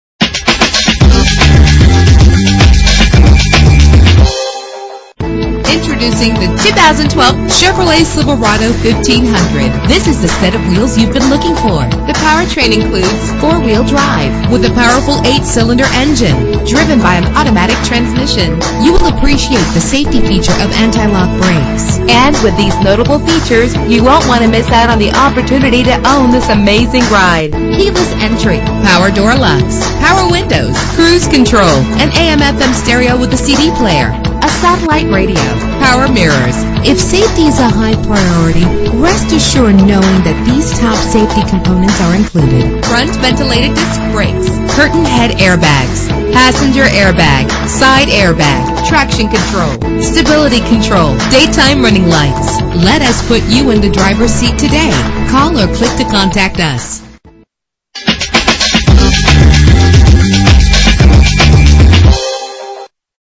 2012 Chevrolet Silverado 1500 - Cleveland OH
Year: 2012
Make: Chevrolet
Model: Silverado 1500
Trim: LS
Engine: 4.8 liter 8 cylinder 16 valve
Transmission: 4-Speed Automatic
Color: Blue Granite
Mileage: 2
Address:
15315 Lorain Avenue